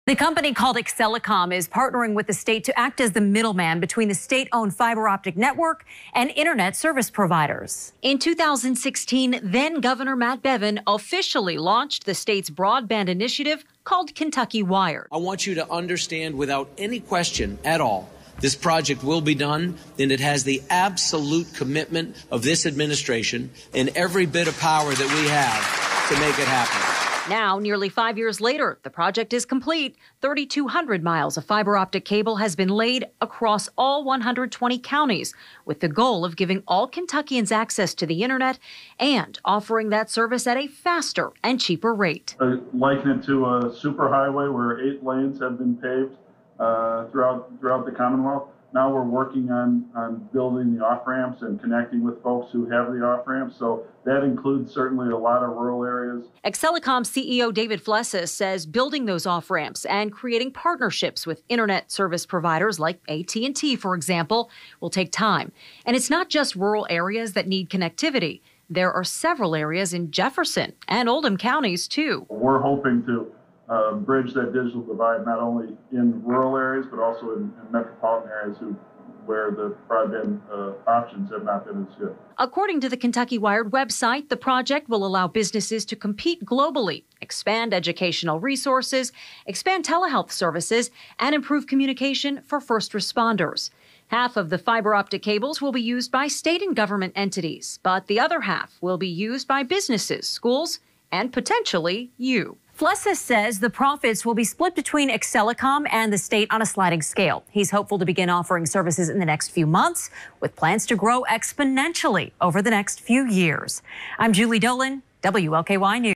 Project brings 3,200 miles of fiber optic cable across KY: What it means for your internet at home
After years of delays, the KentuckyWired project is complete, and now, a new Louisville-based company will serve as the wholesale partner to the commonwealth.